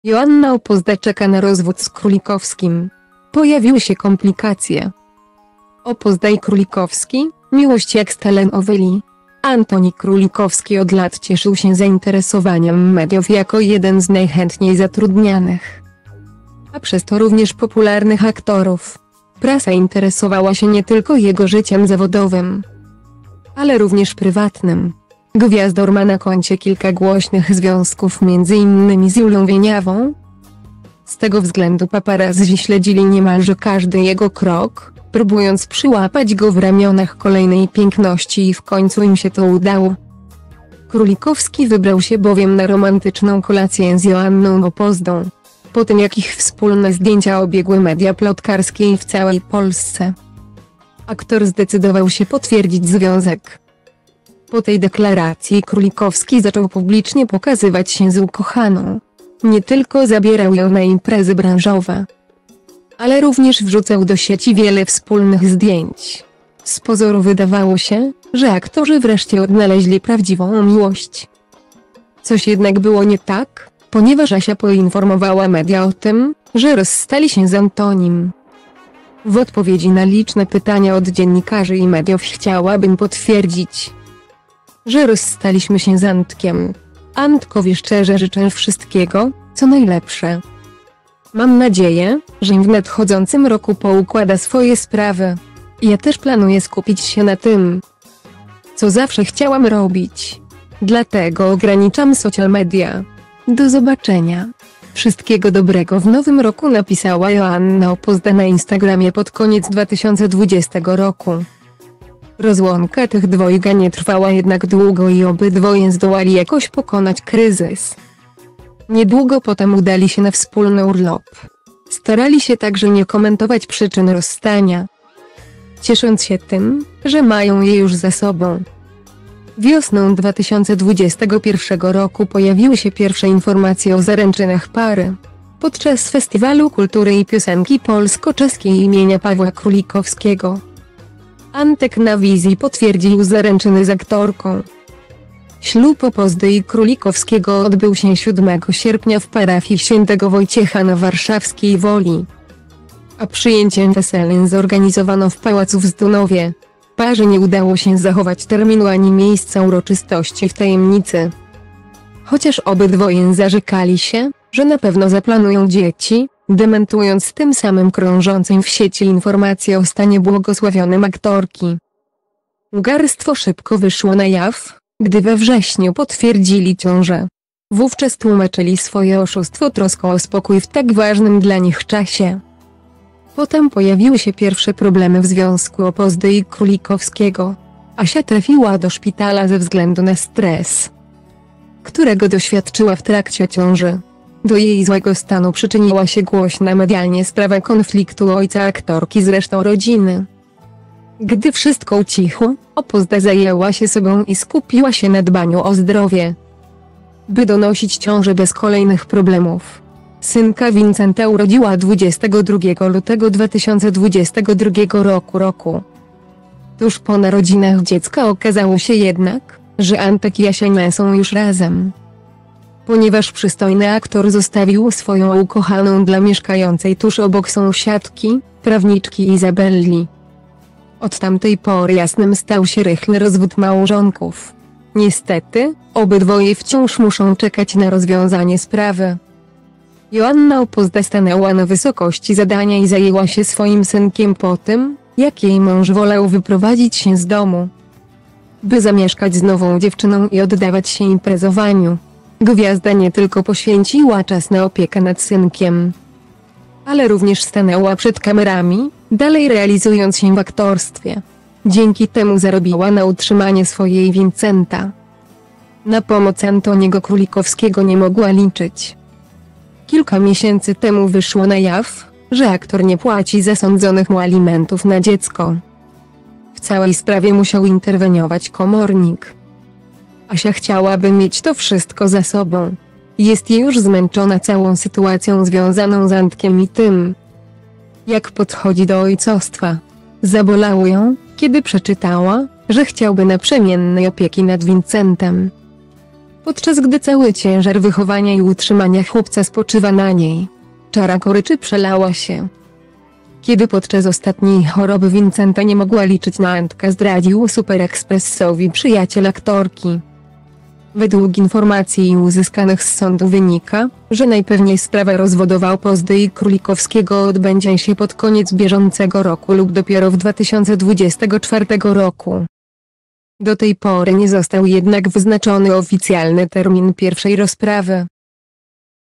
Joanna Opozda czeka na rozwód z Królikowskim. Pojawiły się komplikacje!
Opozda i Królikowski - miłość jak z telenoweli? Antoni Królikowski od lat cieszył się zainteresowaniem mediów jako jeden z najchętniej zatrudnianych,a przez to również popularnych aktorów. Prasa interesowała się nie tylko jego życiem zawodowym,ale również prywatnym. Gwiazdor ma na koncie kilka głośnych związków m. in. z Julią Wieniawą.